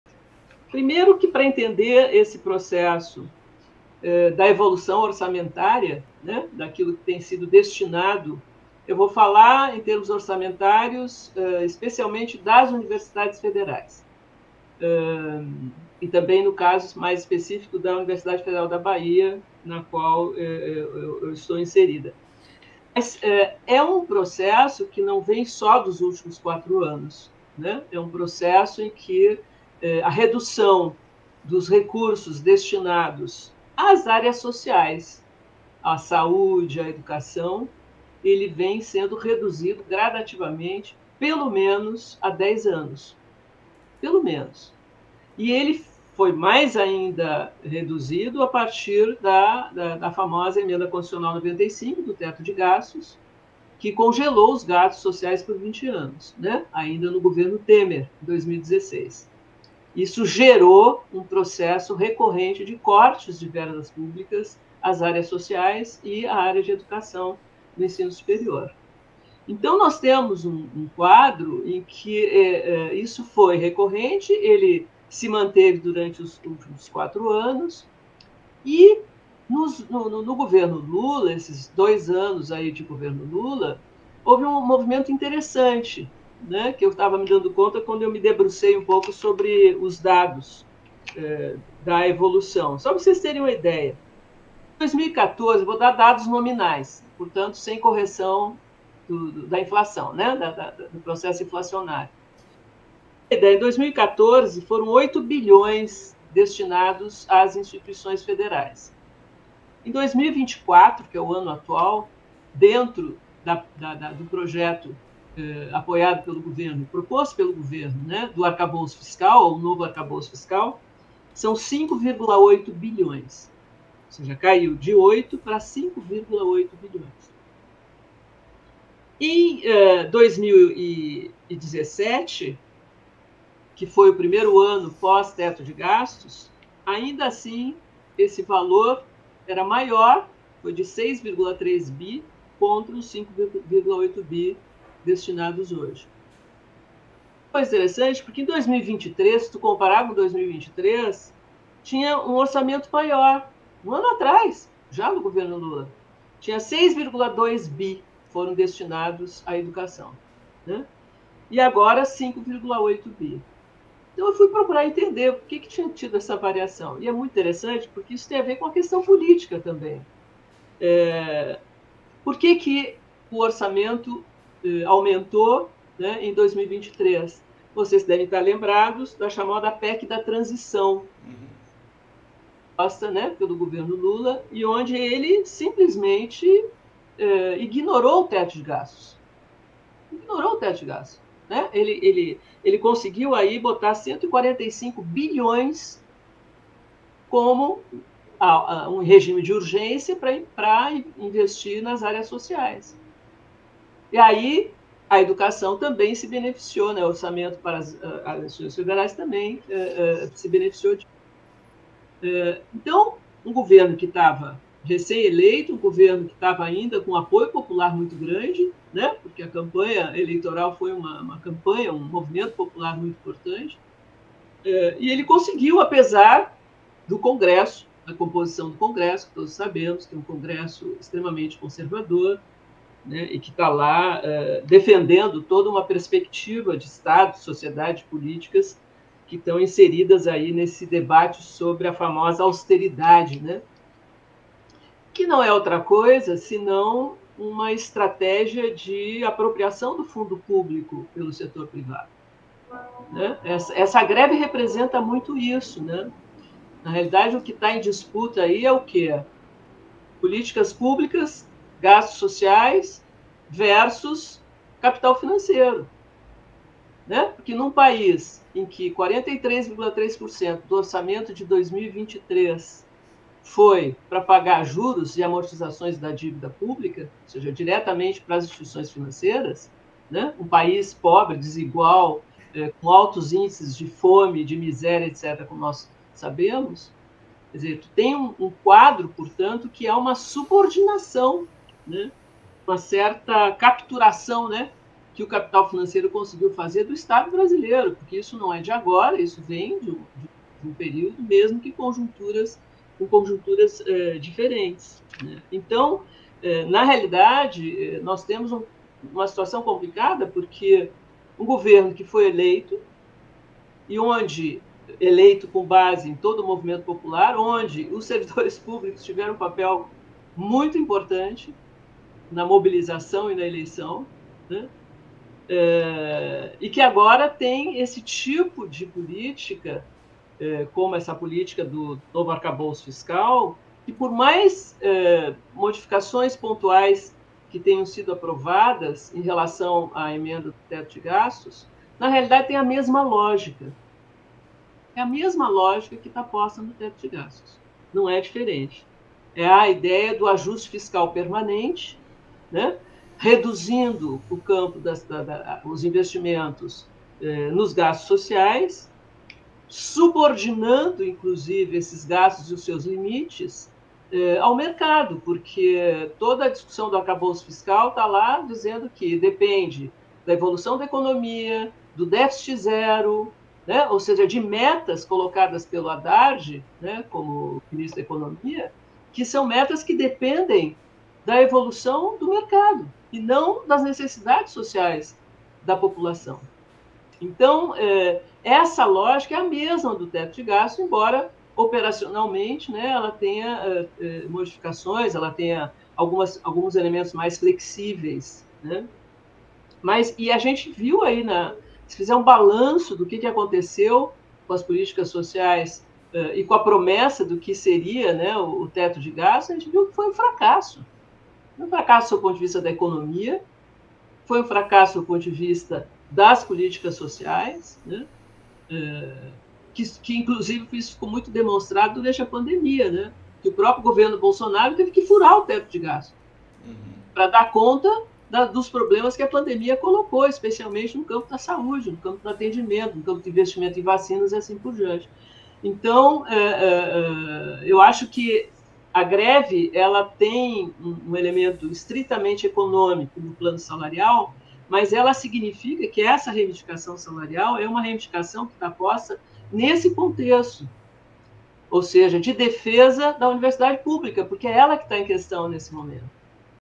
A greve e a redução do orçamento das universidades federais
A greve nas universidades e institutos federais também representa os limites do arcabouço fiscal em relação aos investimentos necessários para o país. "O que está em disputa é políticas públicas e gastos sociais versus capital financeiro", sintetiza a economista e socióloga Graça Druck, em entrevista ao Outras Palavras TV.

OUTRAS PALAVRAS